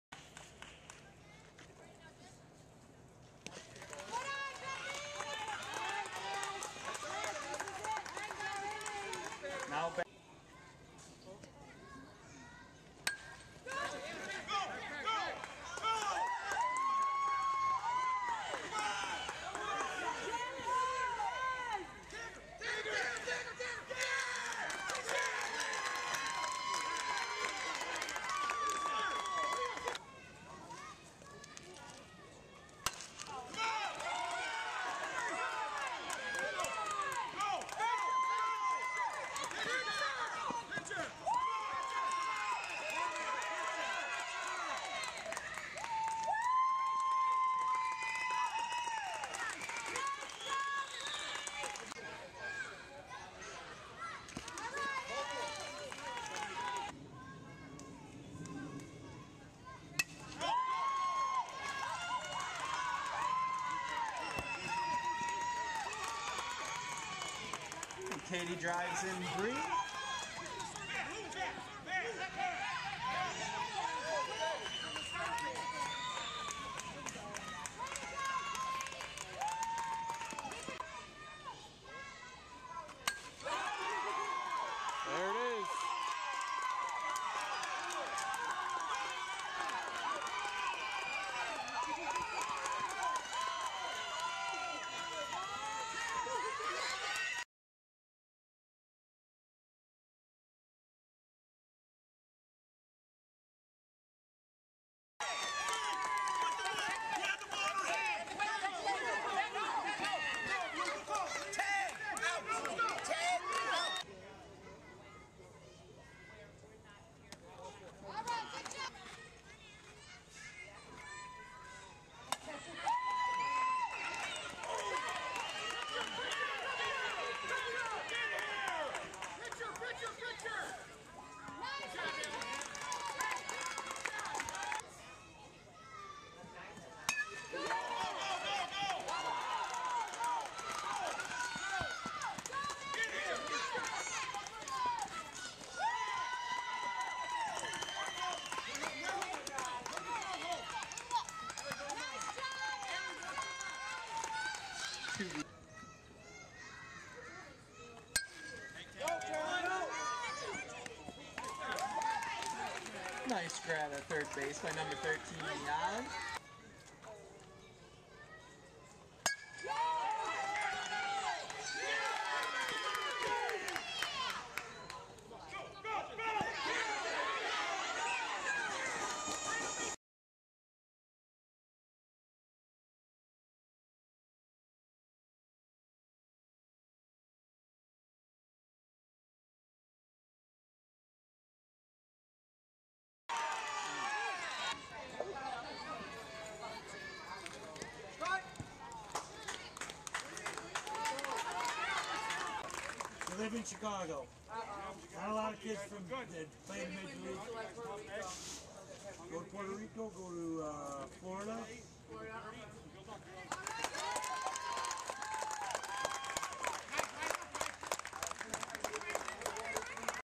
Homewood 7 19
A round of at-bats from Homewood tournament.